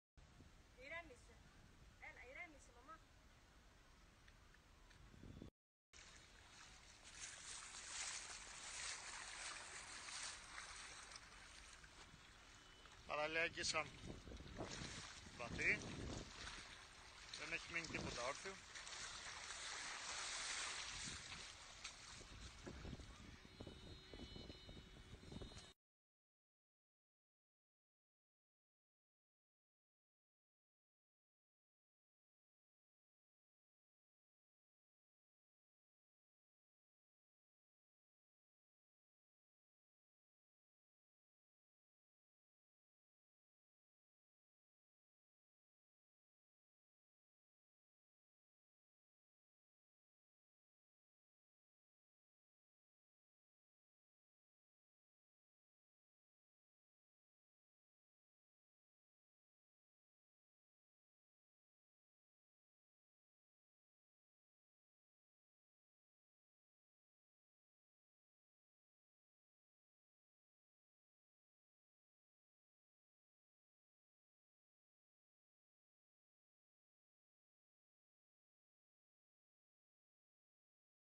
Σεισμός Σάμος, Τσουναμι -Earthquake Samos Island
An earthquake today at 14:00 in Samos, 6,8 richter. A small tsounami followed. 10 peopke are injured.